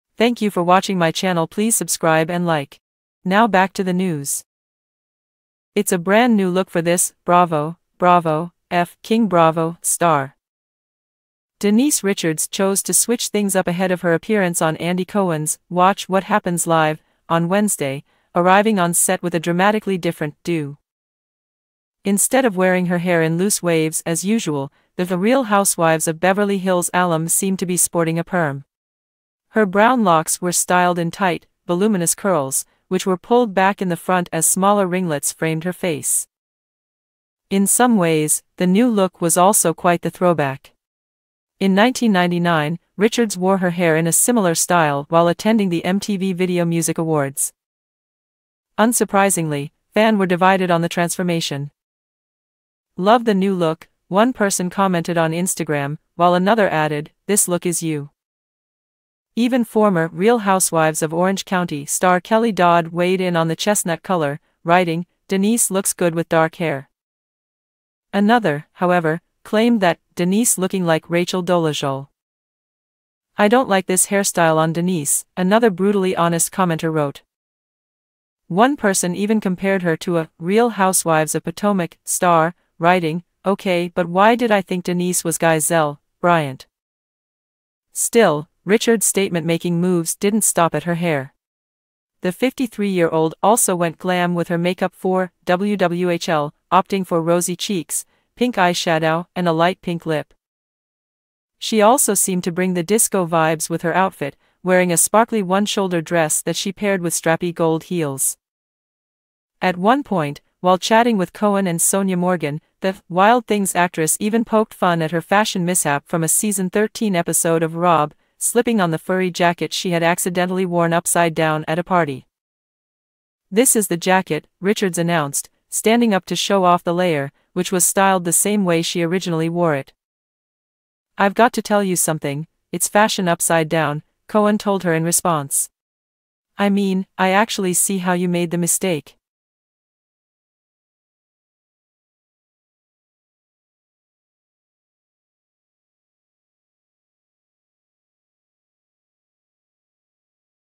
Denise Richards divides fans with curly hair transformation on ‘WWHL’
Denise Richards chose to switch things up ahead of her appearance on Andy Cohen’s “Watch What Happens Live” on Wednesday, arriving on set with a dramatically different ‘do.

Instead of wearing her hair in loose waves as usual, the “Real Housewives of Beverly Hills” alum seemed to be sporting a perm. Her brown locks were styled in tight, voluminous curls, which were pulled back in the front as smaller ringlets framed her face.